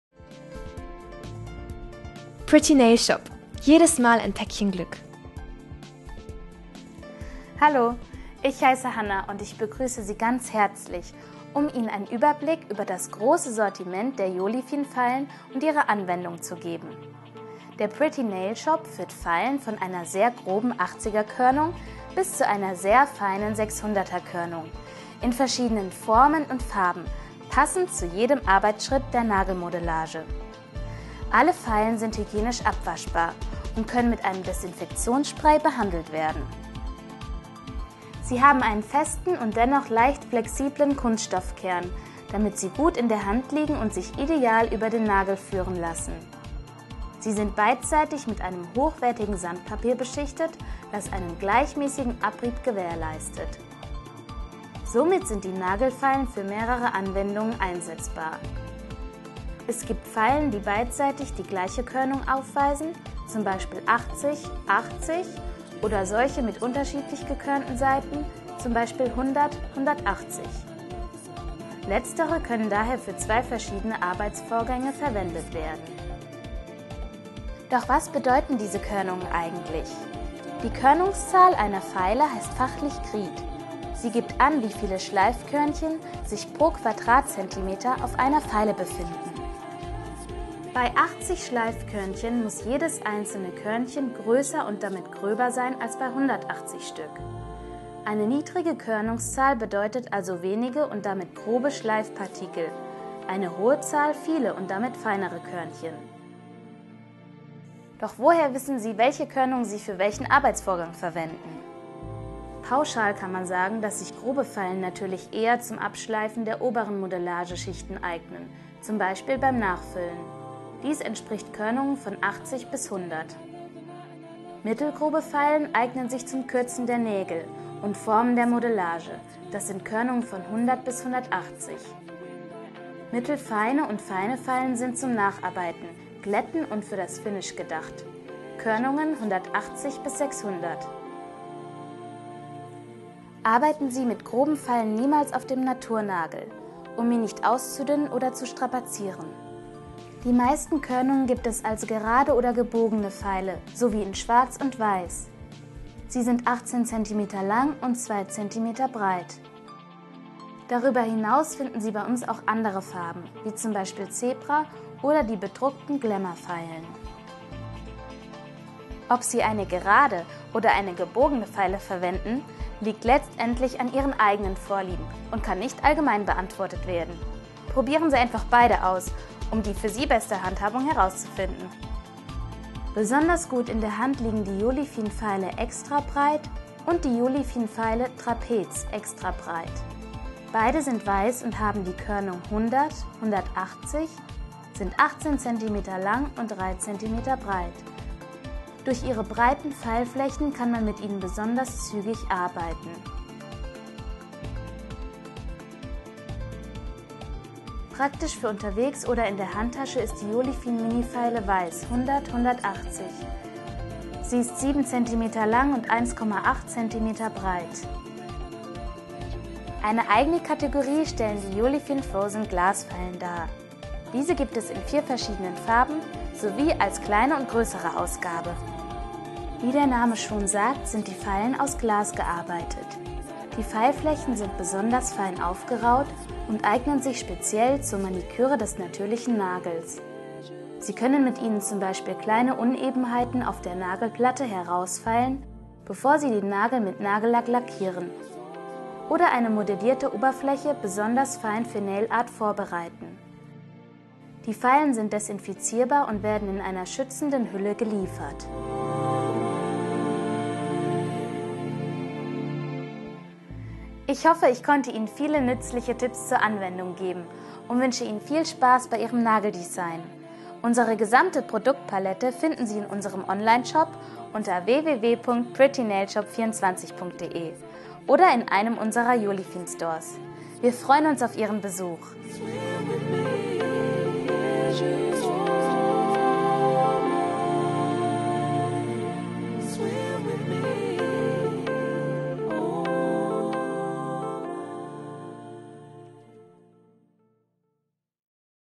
Nagelfeilen für die gelungene Maniküre und künstliche Fingernägel
Ein absolutes Muss für jedes gelungene Nageldesign ist ein optimal befeilter Nagel. Feilen in unterschiedlicher Form und Körnung sind hierfür die besten Helfer.